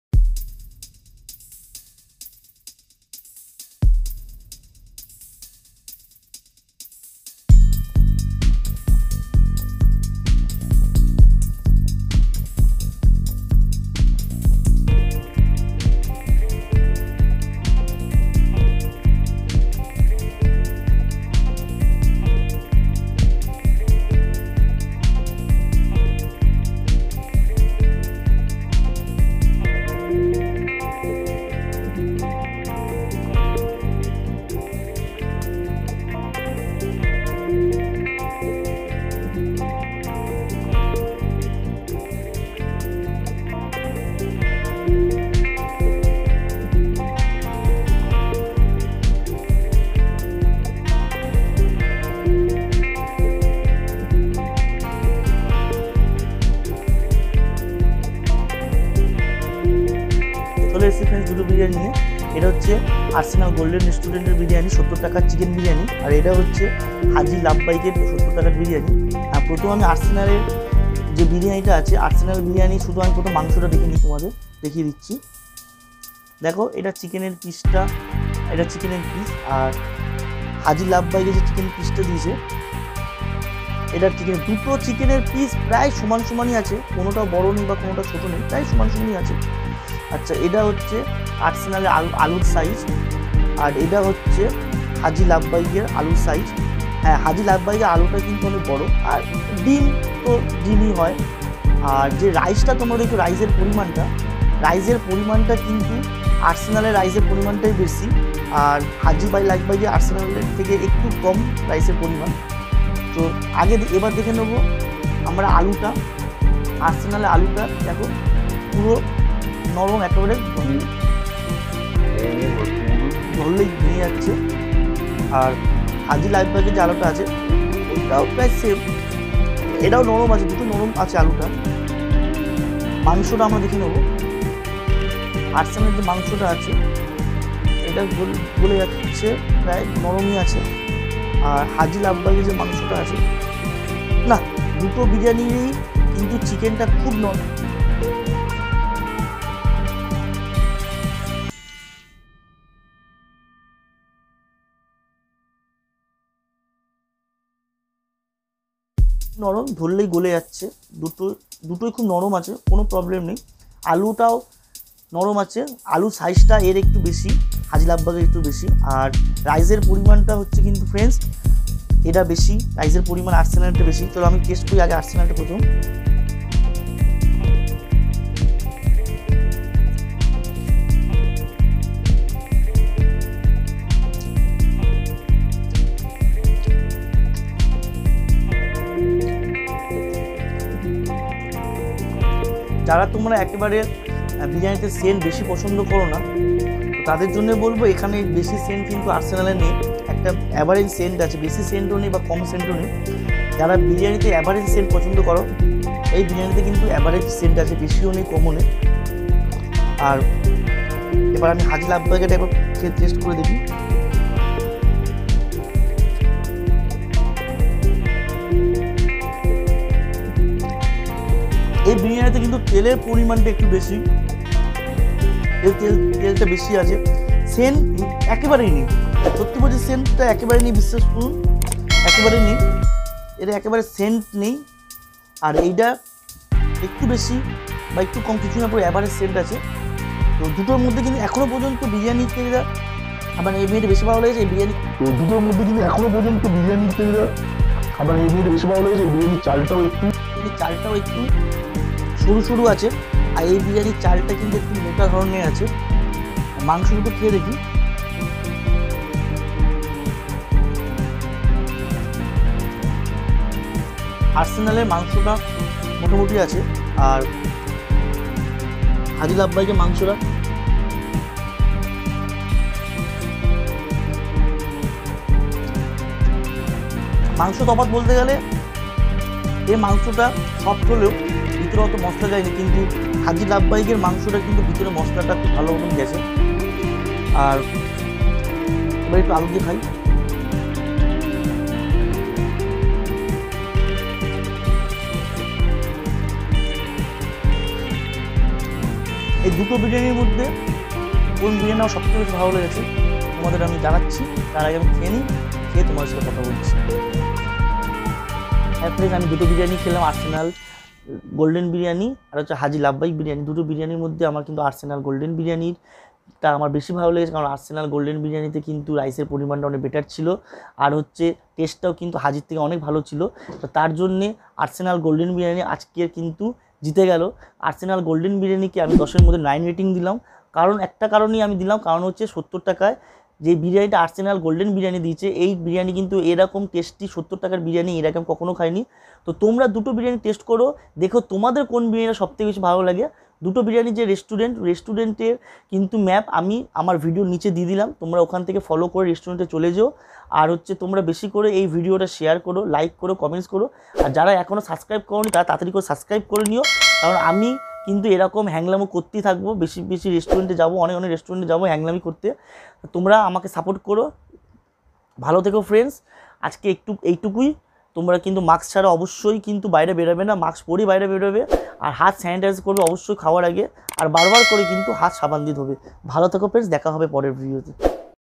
KOLKATA CHEAPEST CHICKEN BIRYANI BATTLE RS 70 VS. 70 | BEST BIRYANI PLACE IN KOLKATA
Hi everyone this is Jeet from Hangla jeet...
On this Channel, I will review all the best food places in Kolkata.